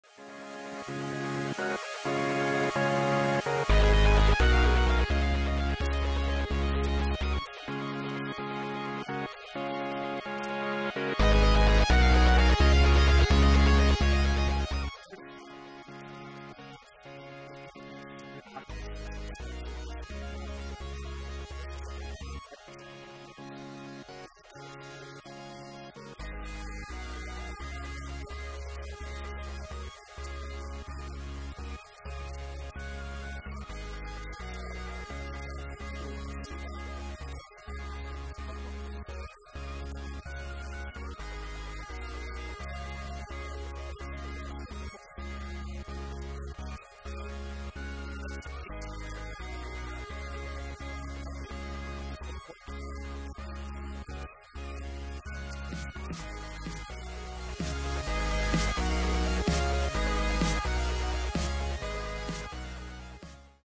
Geoff "iNcontroL" Robinson Interview with GUNNAR
iNcontroL stopped by to chat with the GUNNAR team about why he enjoys wearing GUNNARS